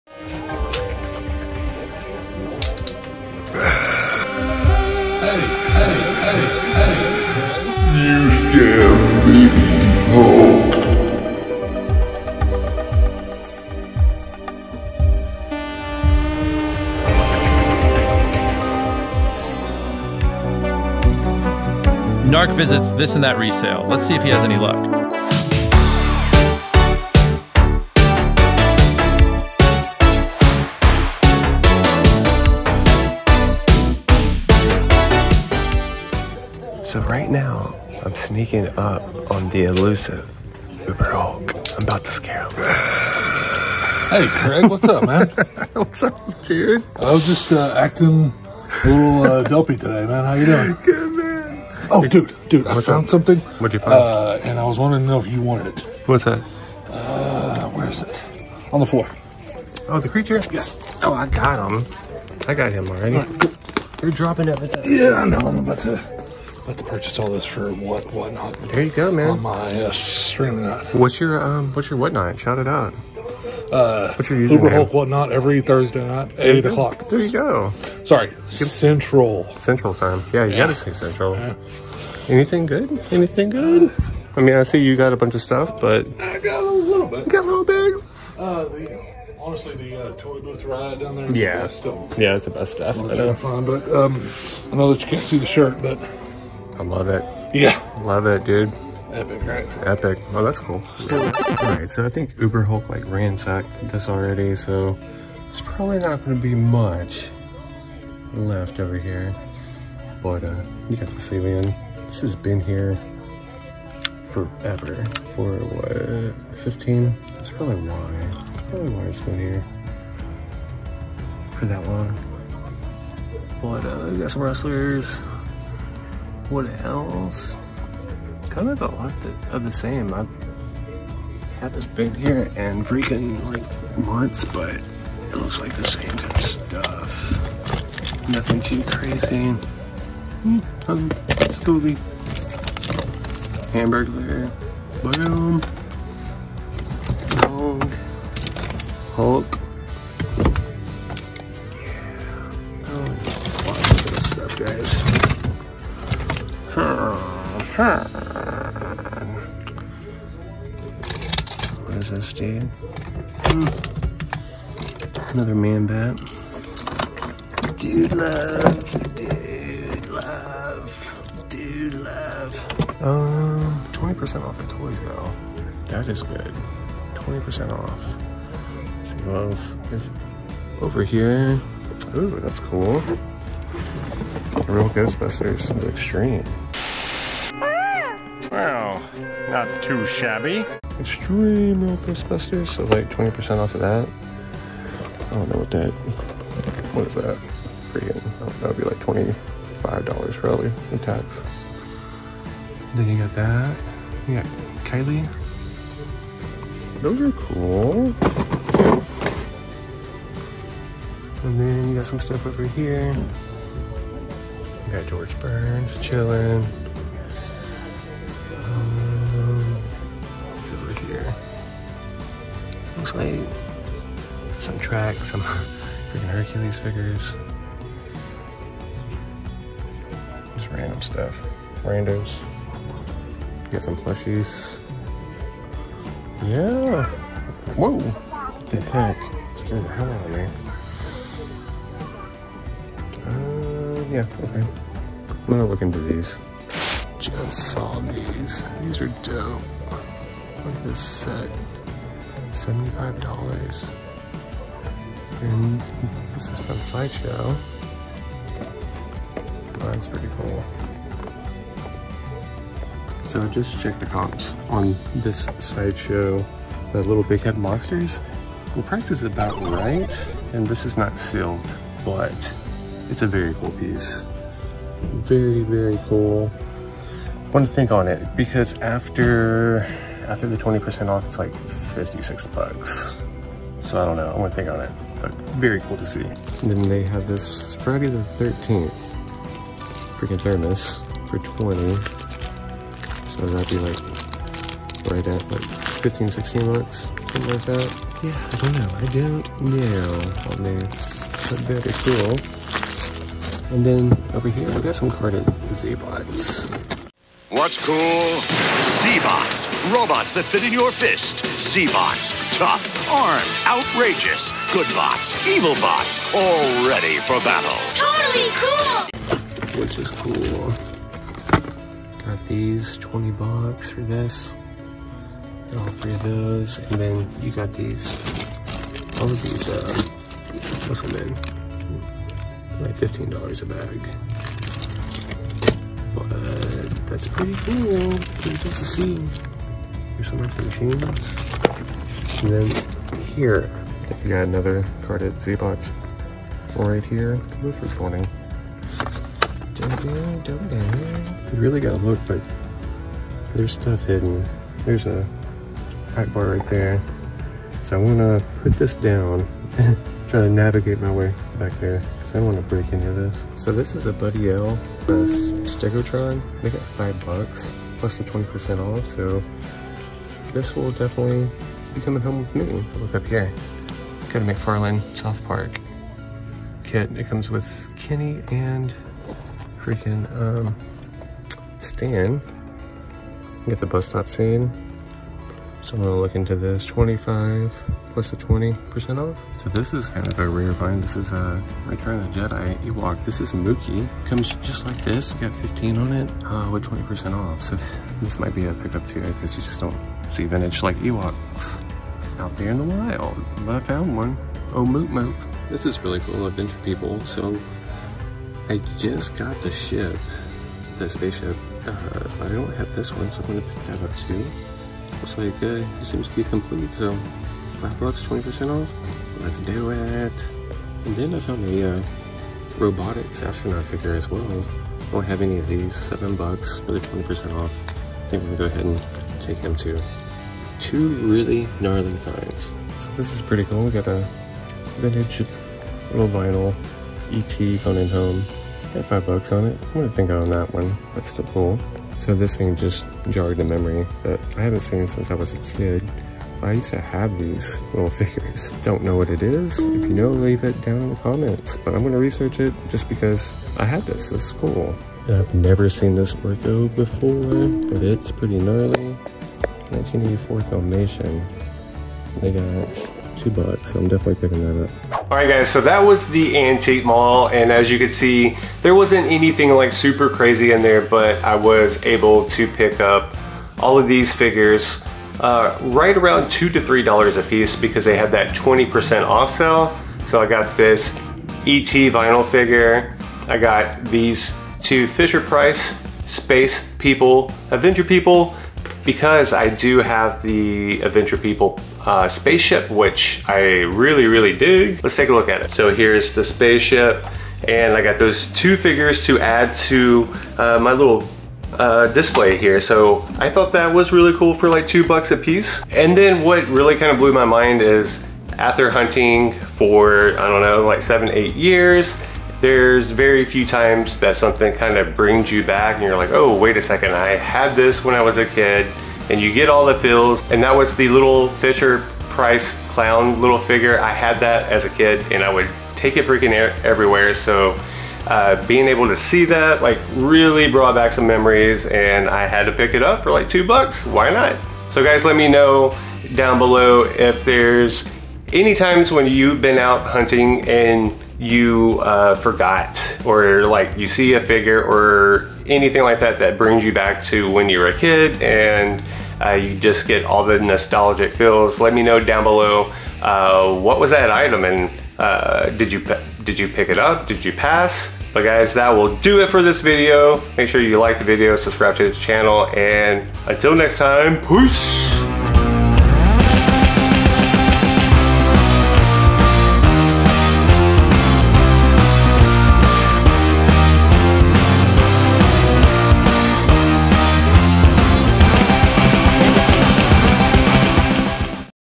Rediscovering a Vintage Action figure from my childhood at this Antique Mall
I go to a local antique mall and find a toy I had as a kid.  I totally forgot about this!

The Syndicate Toy Hunters
The Nerd Bar
The Pence Pack
The Toy Stash